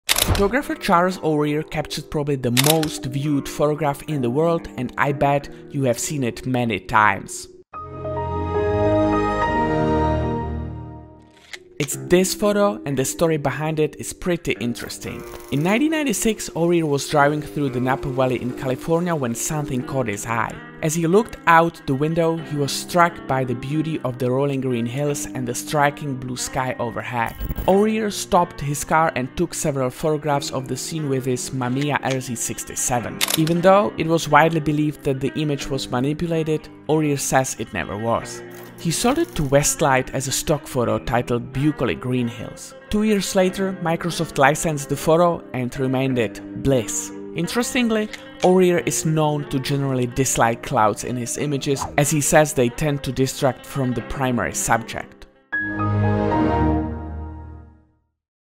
The Story of Microsoft's Bliss
Join us as we delve into the fascinating story behind 'Bliss,' arguably the most viewed photograph in the world, captured by Charles O'Rear. This video takes you on O'Rear's journey through Napa Valley in 1996, where the beauty of rolling green hills and a striking blue sky inspired him to stop and capture the scene with his Mamiya RZ67. Despite beliefs of manipulation, O'Rear maintains the photo's authenticity. Later sold to Westlight and famously licensed by Microsoft, this image became synonymous with Windows XP. Explore the backstory of this iconic wallpaper and discover how O'Rear's preference for cloudless skies played a role in creating this legendary photograph.